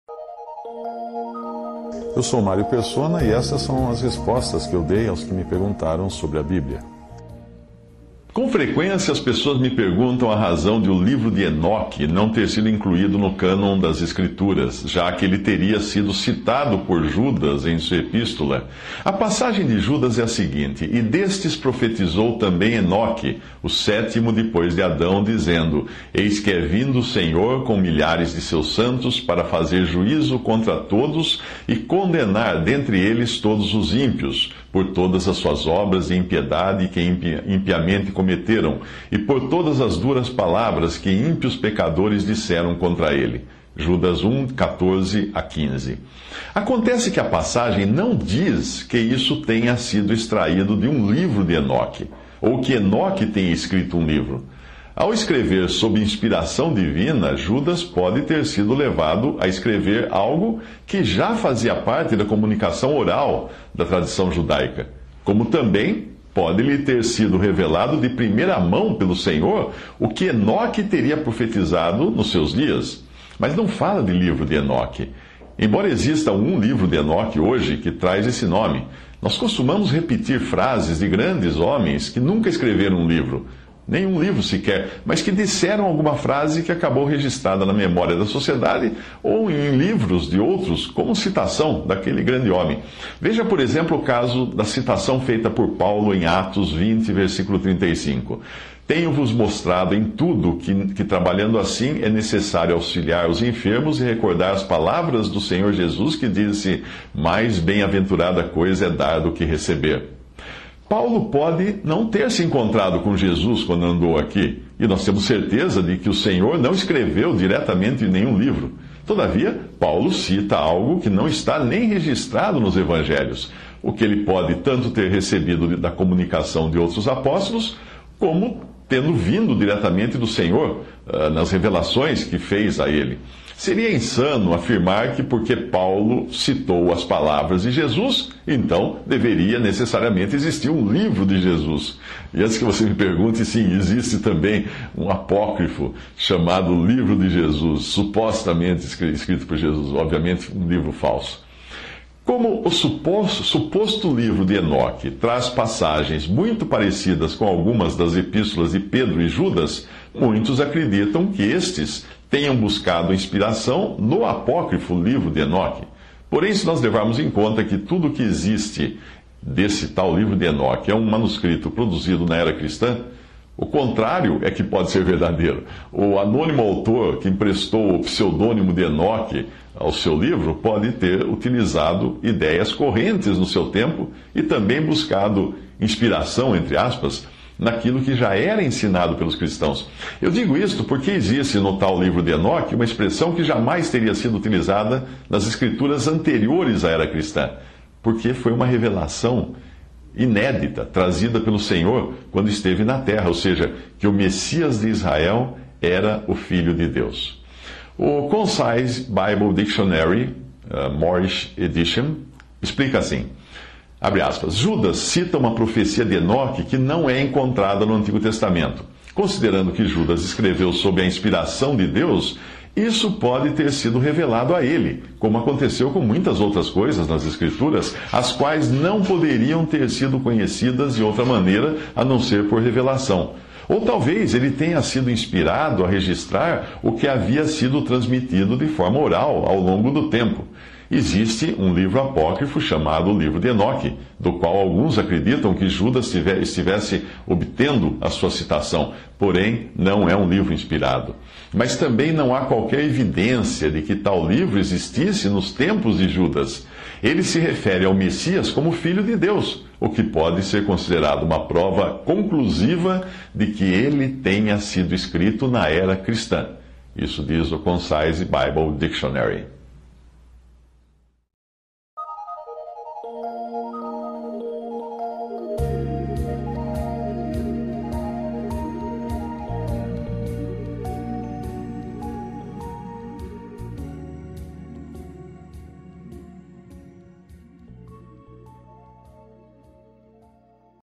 Teria Judas citado o Livro de Enoque?
Com frequência pessoas me perguntam a razão de o "Livro de Enoque" não ter sido incluído no cânon das Escrituras, já que ele teria sido citado por Judas em sua epístola. A passagem de Judas é: "E destes profetizou também Enoque, o sétimo depois de Adão, dizendo: Eis que é vindo o Senhor com milhares de seus santos; para fazer juízo contra todos e condenar dentre eles todos os ímpios, por todas as suas obras de impiedade, que impiamente cometeram, e por todas as duras palavras que ímpios pecadores disseram contra ele." (Jd 1:14-15)